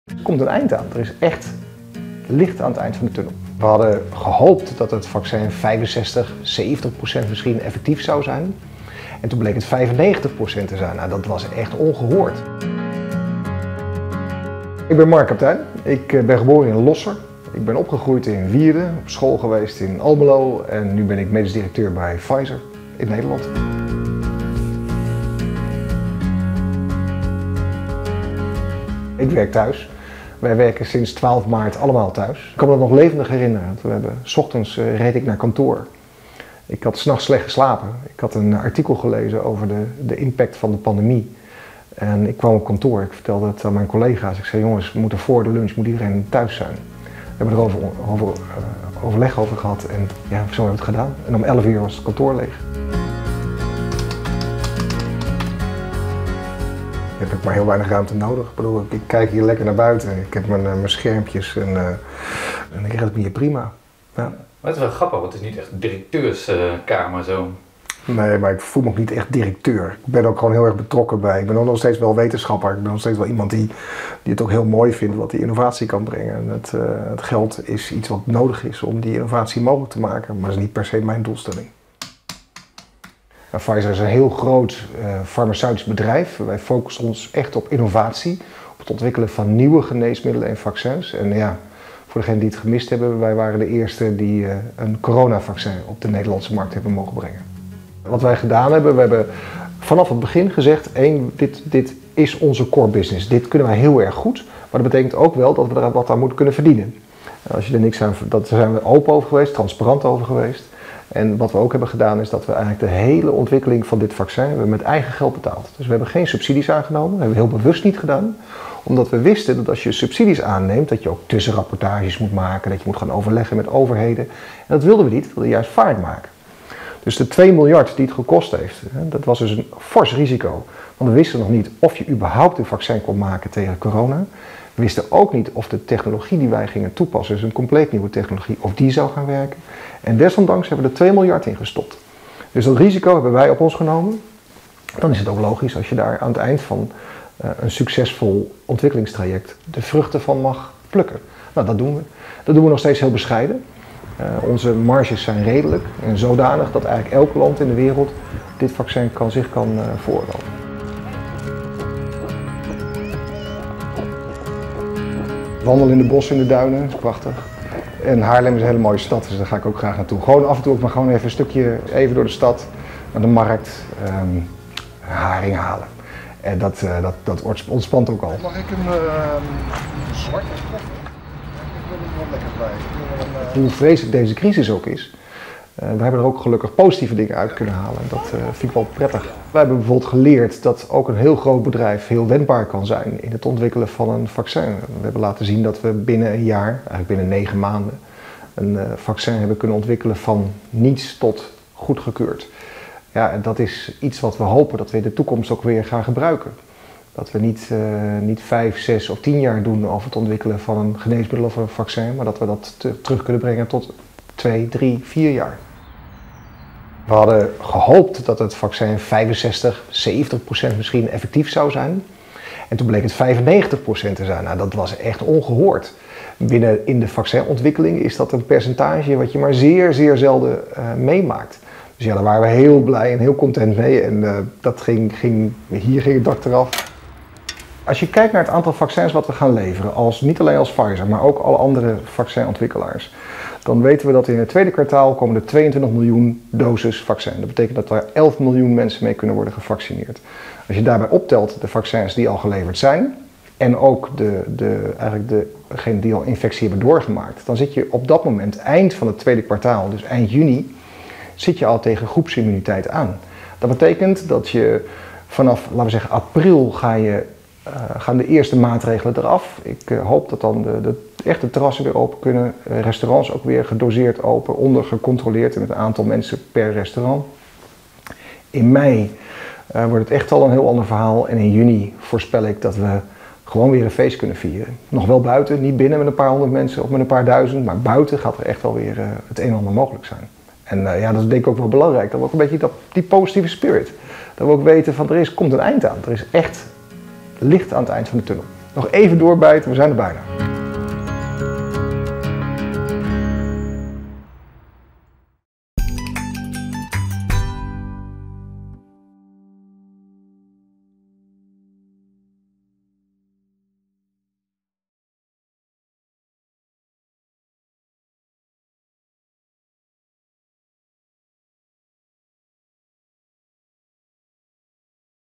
Pfizer-directeur Marc is positief verrast over bescherming vaccin | RTV Oost
Pfizer-directeur Marc Kaptein is het afgelopen jaar positief verrast over de bescherming die het vaccin biedt. Eerst werd gedacht dat de bescherming rond de 50% zou liggen.

In deze vijfdelige portretreeks blikken hoofdpersonen rondom de coronacrisis terug op een jaar corona. Lees hier meer over Marc Kaptein